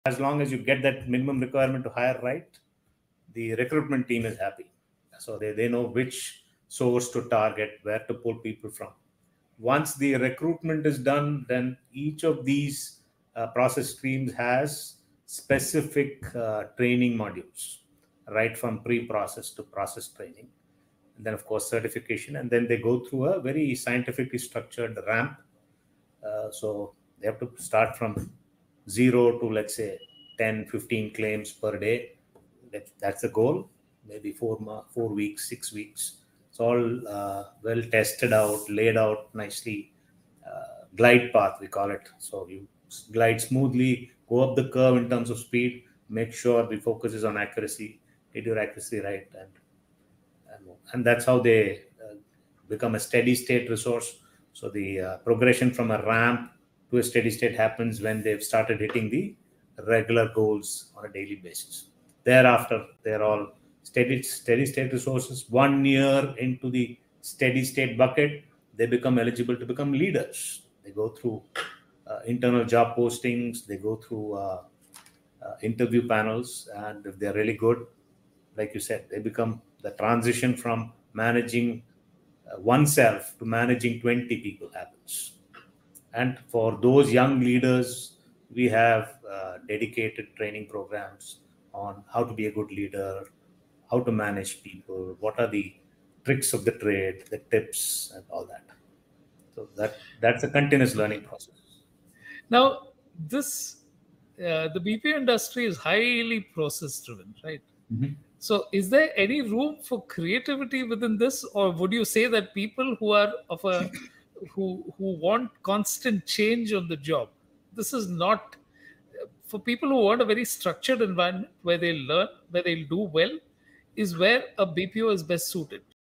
Becoming a Leader in the BPO Industry: Progression, Training, and Room for Creativity
Discover the path to leadership in the BPO industry, from starting as a ramp resource to becoming a steady state expert. Learn about the training programs and internal job postings that pave the way for young leaders. Find out if the BPO industry is the right fit for those seeking a structured yet creative environment.